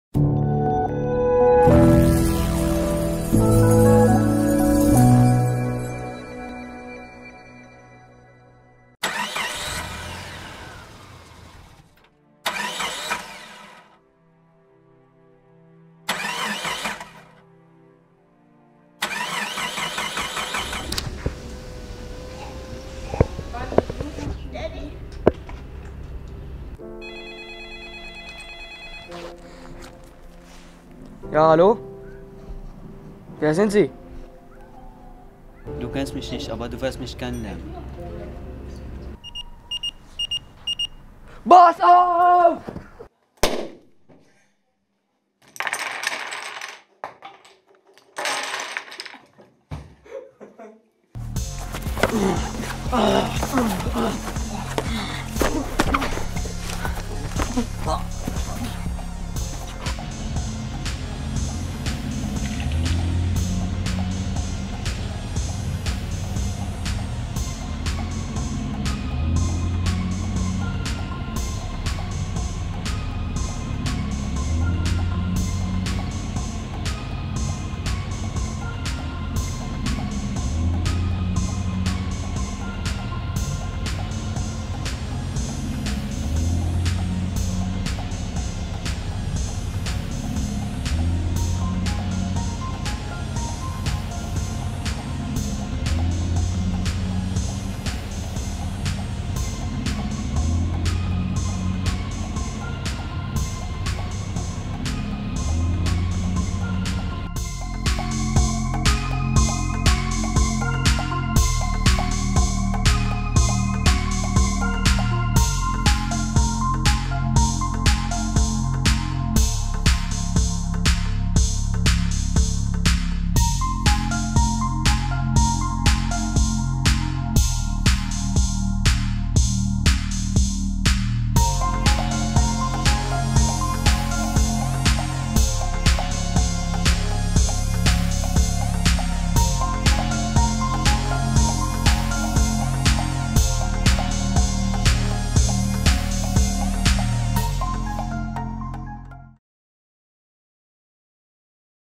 "Fast & Furious 7" geschwedet – reMAKE your Movie II | WTV
Dieser Film entstand im Projekt reMAKE your Movie II - Lieblingsfilme mit einfachen Mittel neu inszenieren.